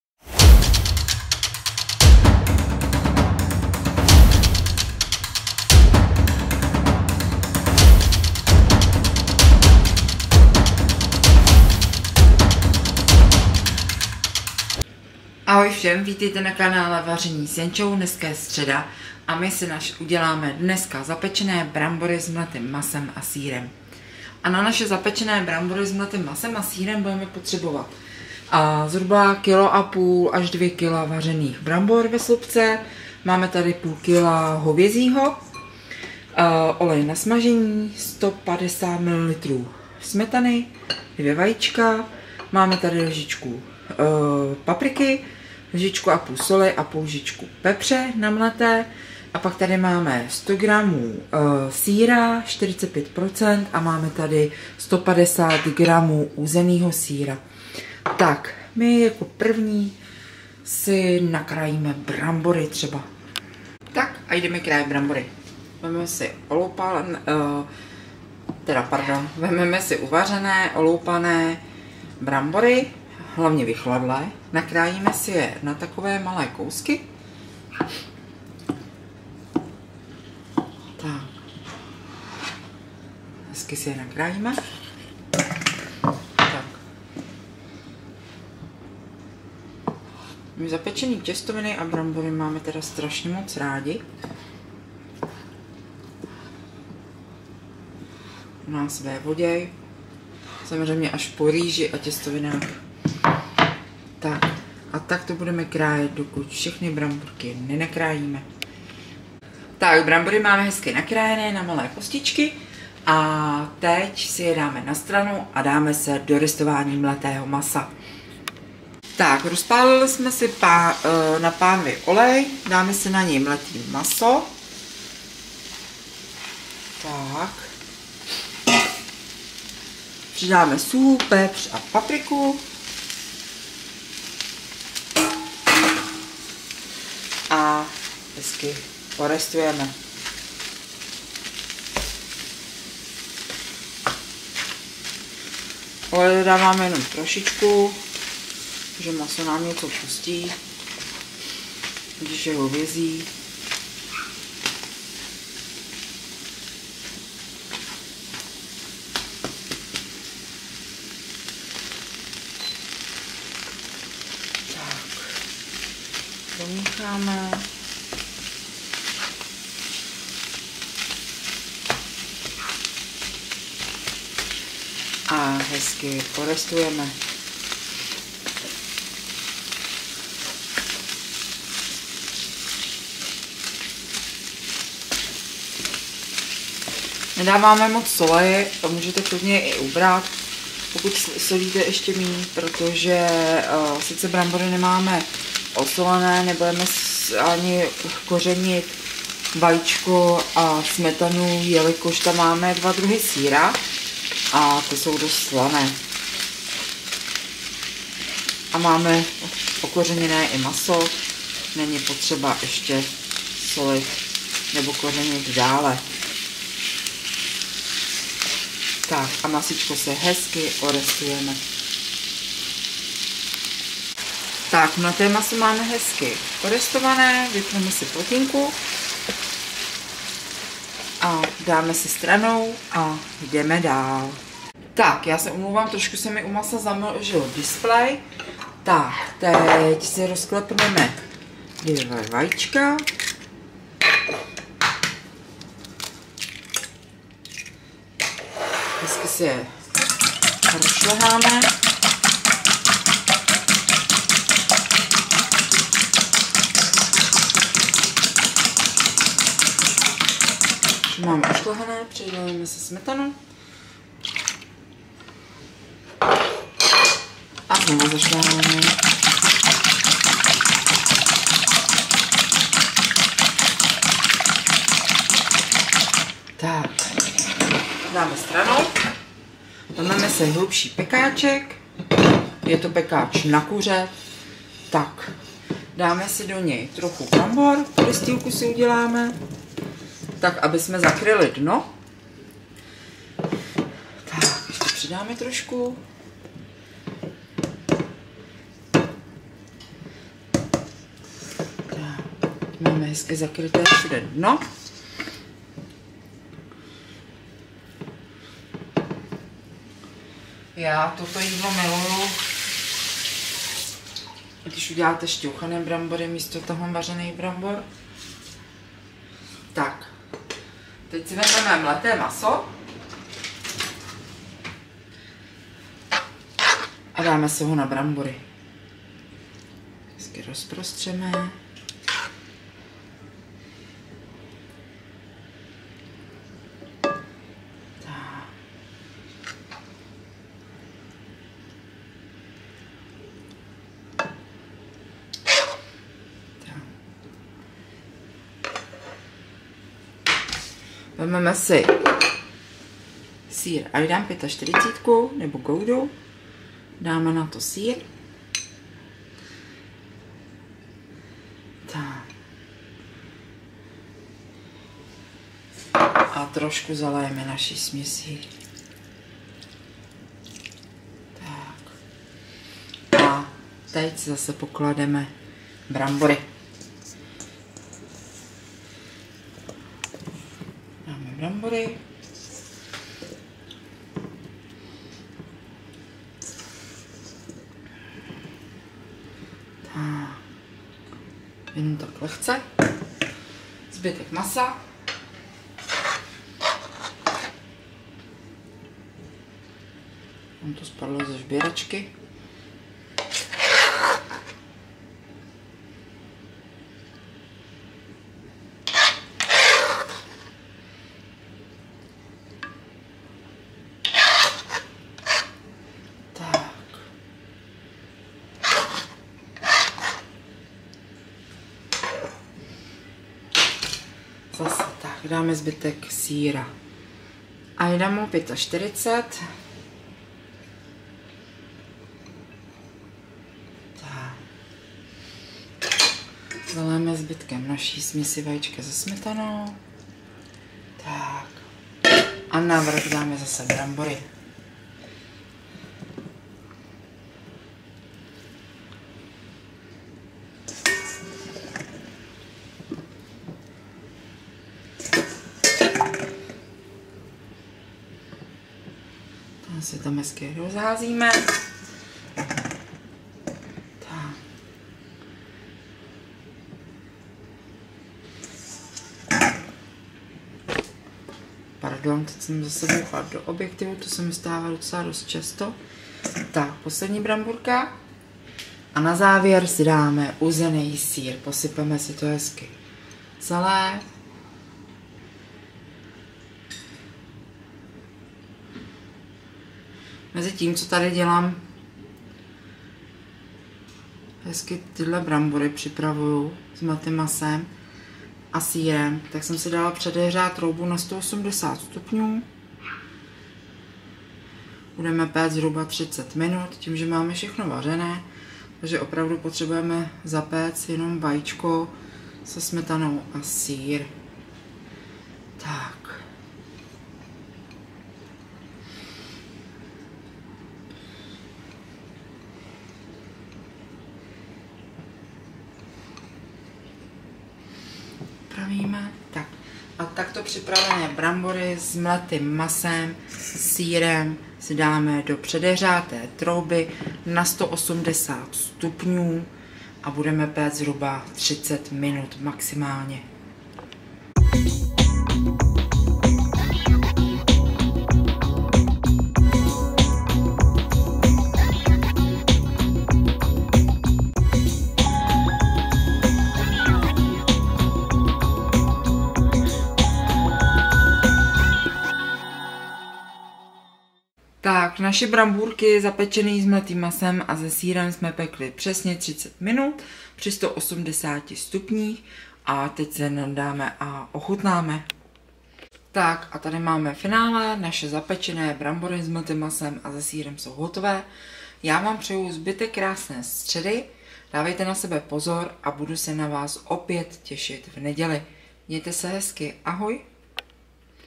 Zapečené brambory s mletým masem
Na zapečené brambory potřebujeme:
1,5 - 2kg vařených brambor   40 Kč
0,5kg hovězí mleté maso   80 Kč
150g uzeného sýra    32 Kč
100 eidamu  45%   22 Kč
150ml smetany   12 Kč
2 vejce   4 Kč
1 lžička papriky    2 Kč
0,5 lžičky pepře    1 Kč
1,5 lžičky sole   1 Kč
olej   1 Kč
Celkem:   195 Kč